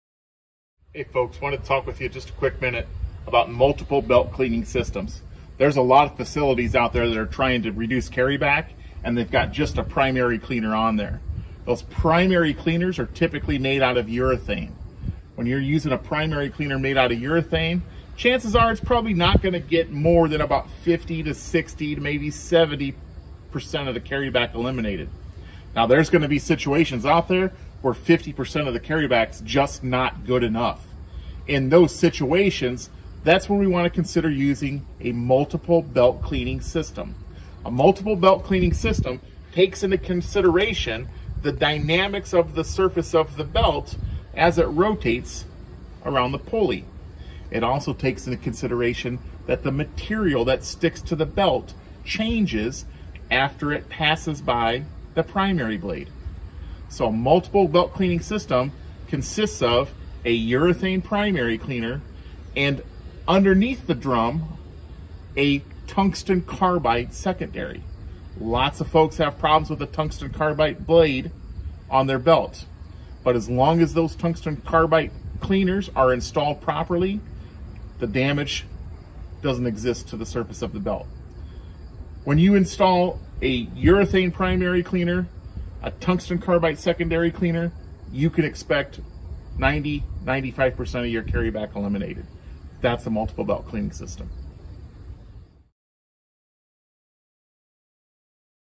Multiple Belt Cleaning Systems
Hear how a multiple-belt-cleaner system effectively tackles carryback.